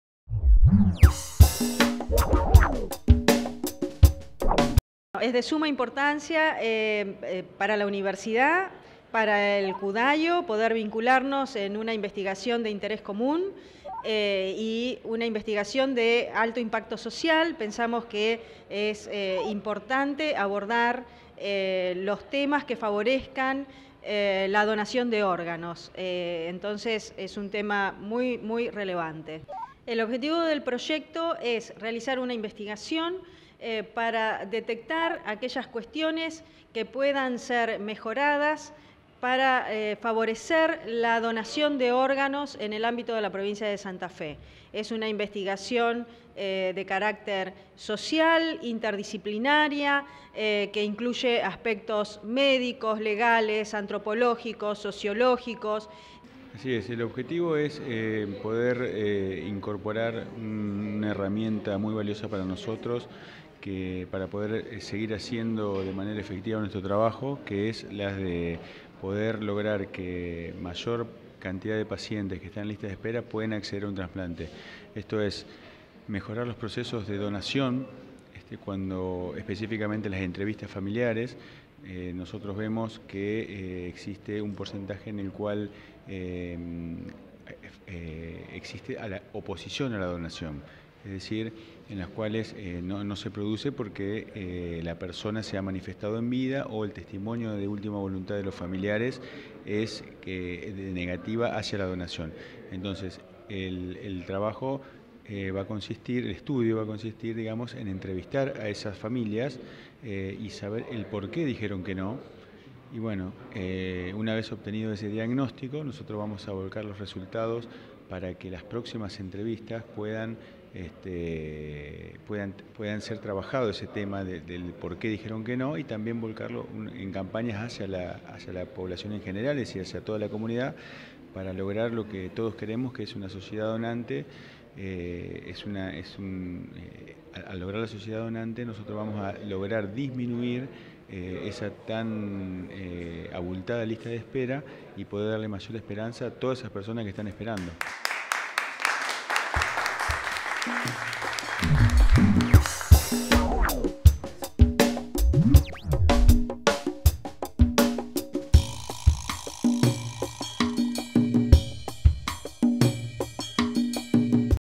Convenio Unr - Cudaio
Se presentó proyecto de investigación sobre la donación de órganos entre la Universidad Nacional de Rosario y el Cudaio.